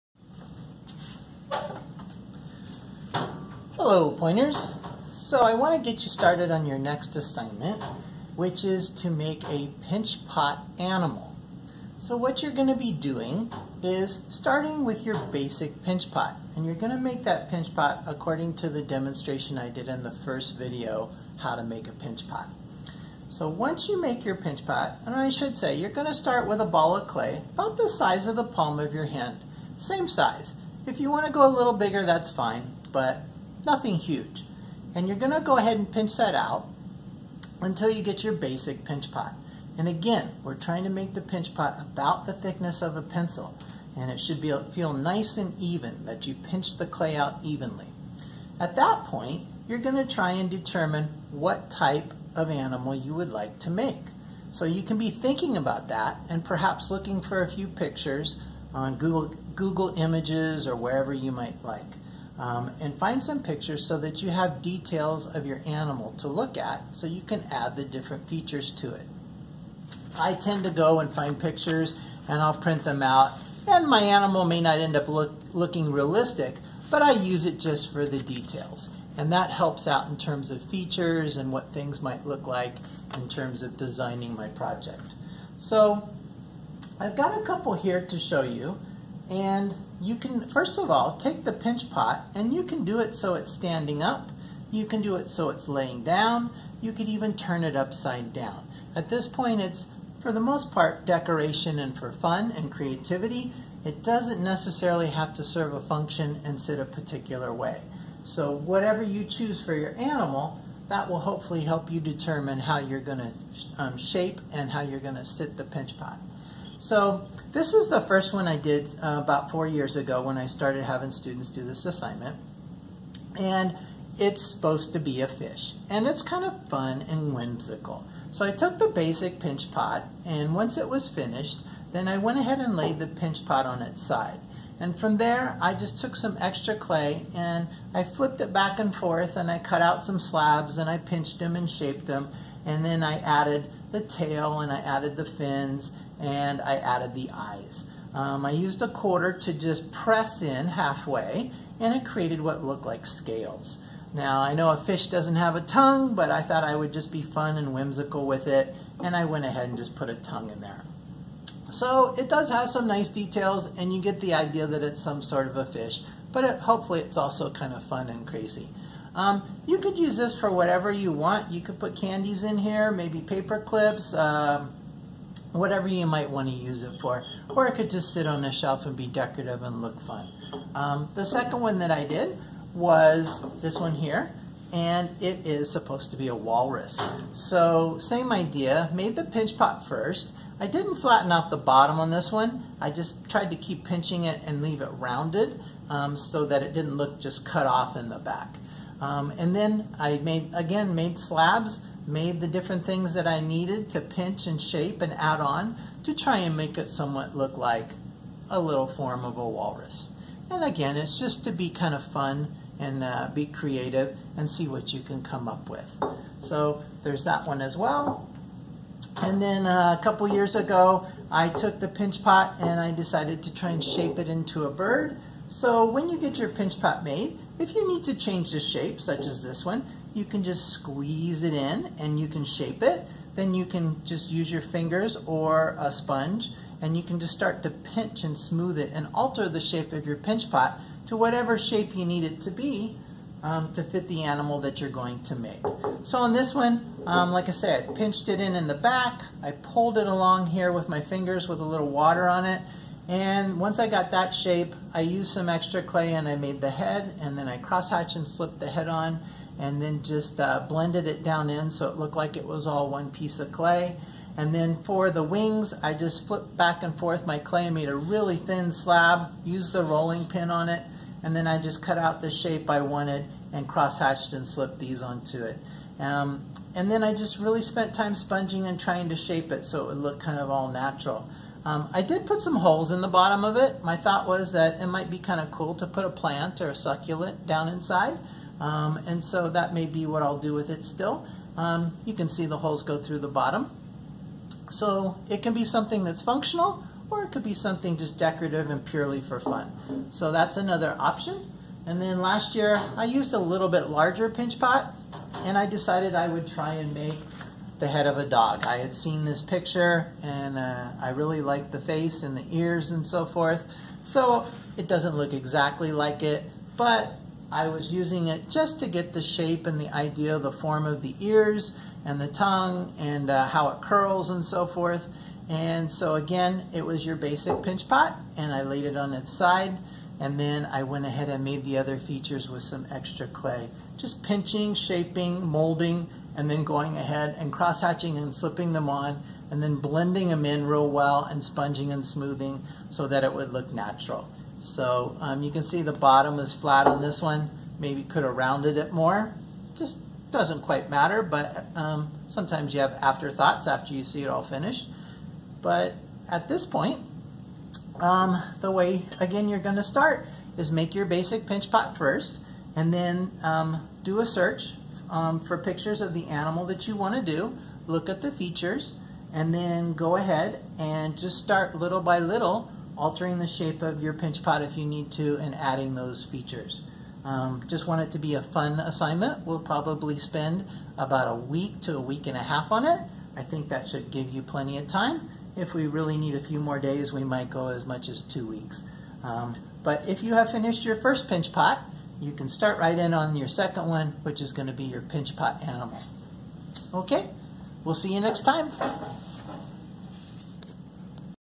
Pinch Pot Animal Assignment & Examples
Brief instruction and showing examples of Pinch Pot Animal assignment.